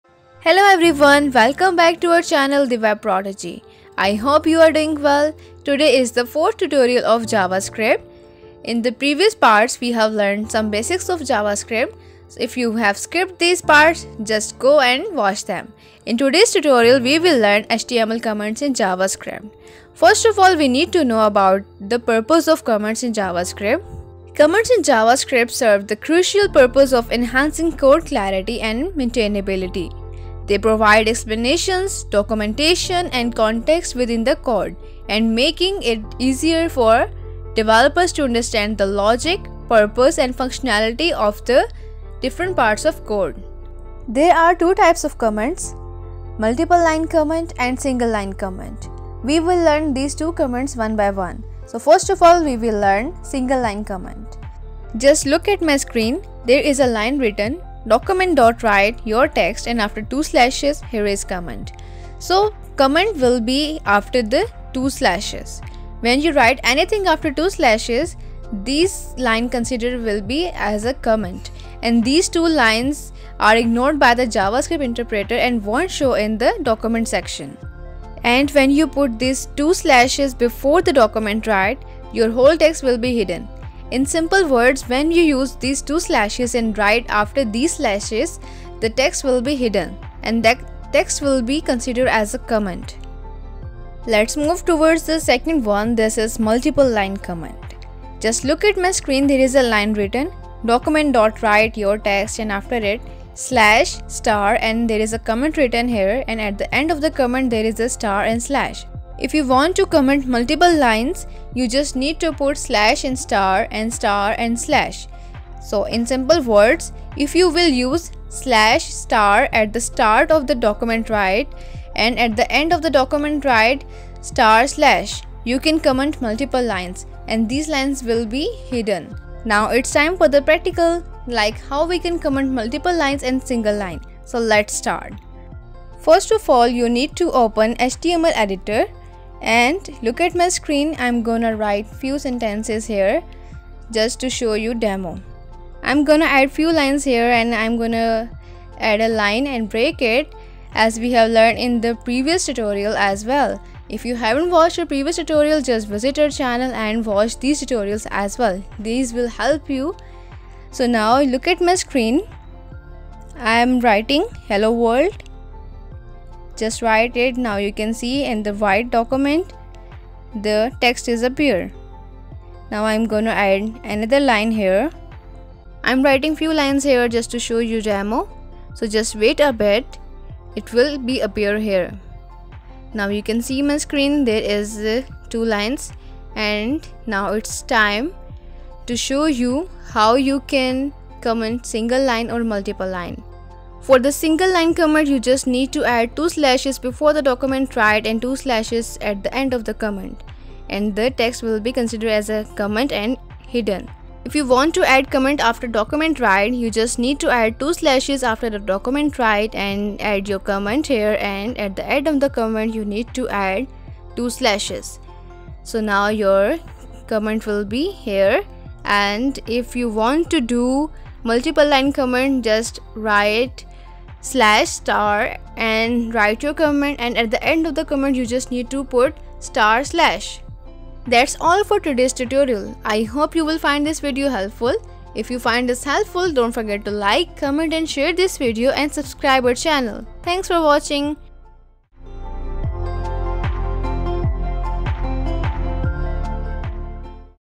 JavaScript Comments Tutorial | Full Overview | The Web Prodigy | Part 4
🚀 Welcome to "The Web Prodigy" tutorial! In this video, you'll discover the power of using comments in JavaScript.

🔍 Video Overview:

Learn the fundamentals of JavaScript comments.
Explore different types of comments (single-line and multi-line) and when to use them.
Understand the importance of documenting your code for future reference and teamwork.
Discover best practices for writing clear and concise comments.
See practical examples of comments in action.

Comments in JavaScript are essential for explaining your code to others and your future self.
Single-line comments start with "//" and are great for brief explanations.
Multi-line comments use "/" and "/" and are perfect for longer explanations or for commenting out blocks of code.
Keep your comments concise, relevant, and up-to-date.
Collaborative coding becomes more manageable when everyone understands your code's intentions.

👩‍💻 Ready to enhance your coding skills? Watch this tutorial and become a JavaScript commenting pro! Let's level up your web development game together! 🚀